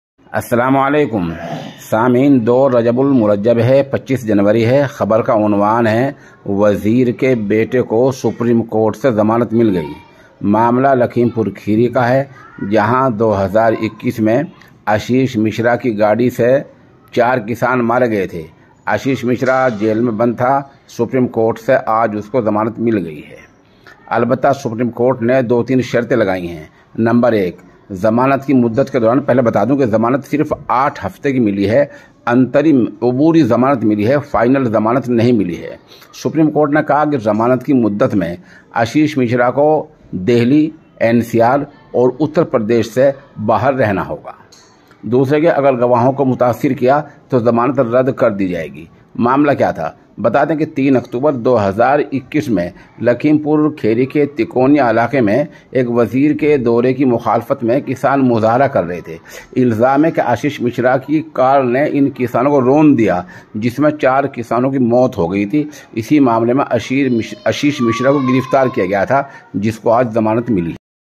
وزیر کے بیٹے کو ضمانت مل گئی Ashish Mishra
मंत्री पुत्र को सुप्रीम कोर्ट से जमानत मिल गई